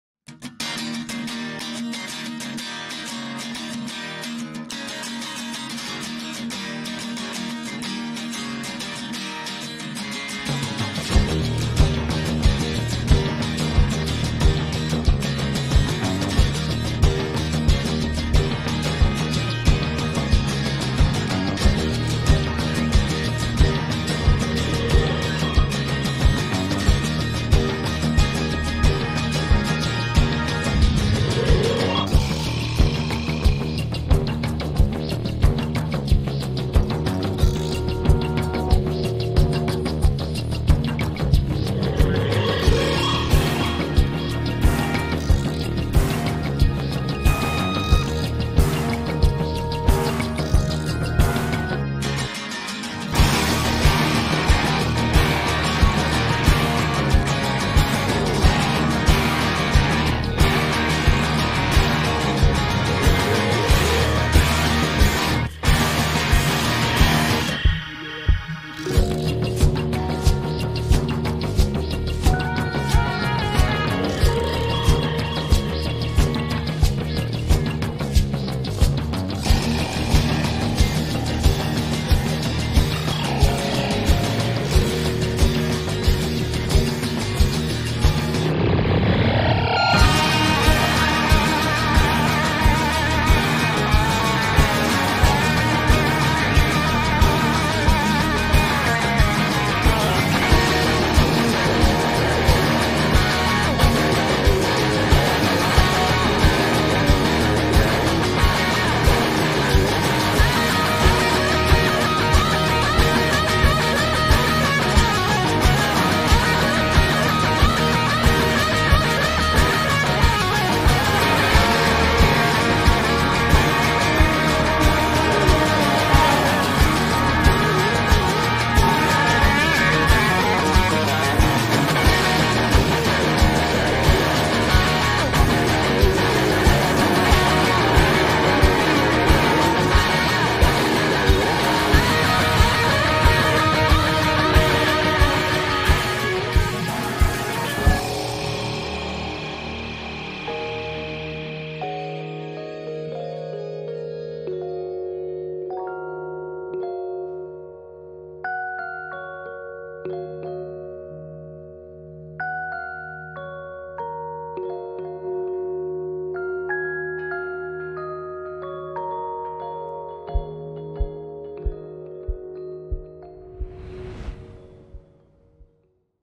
GoPro Quick Story of the weekend
Black Rock Overland went up into the High Sierras to scout out primitive campsites. It did rain on us a bit, but the weather made the trip even more fun.